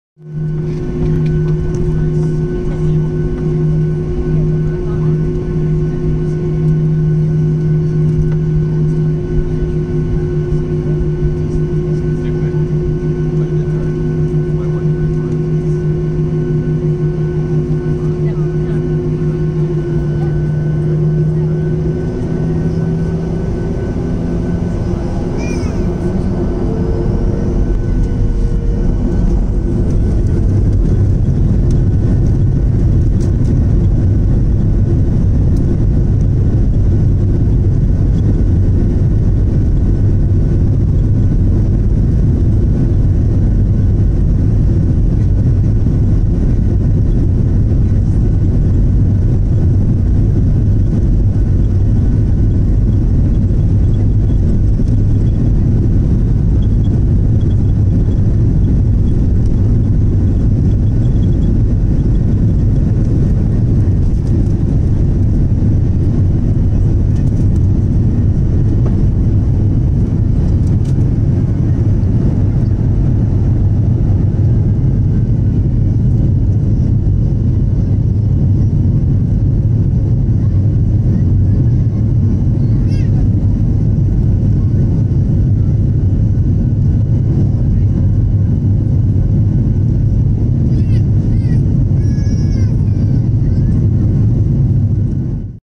[2019] Calgary Takeoff | WestJet 737-600
Departure Runway: 17R
Departure Time: 13:47 MDT

Airline: WestJet
Flight: WS 127
Route: Calgary (YYC) - Vancouver (YVR)
Aircraft: Boeing 737-6CT
Registration: C-GWJU
Seat: 11A
Date: 2019/08/20